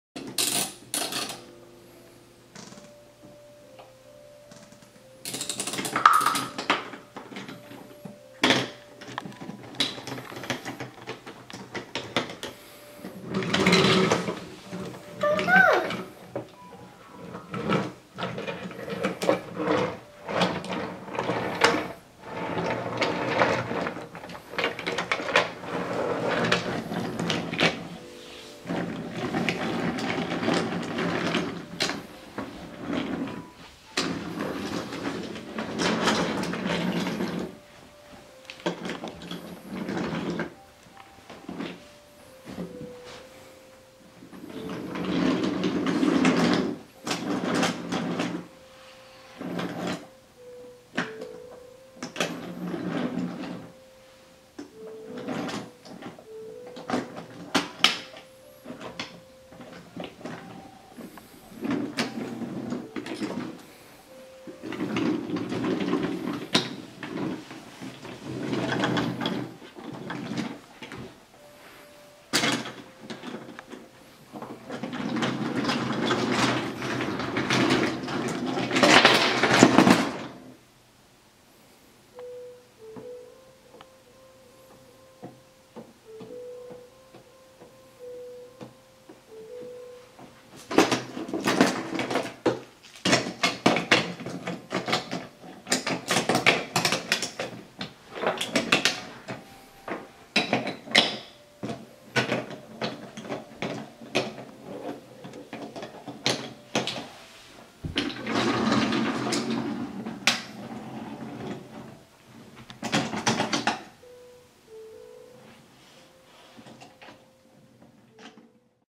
Imaginarium Mountain Rock Train Table
Ryder playing with his Imaginarium Mountain Rock Train Table.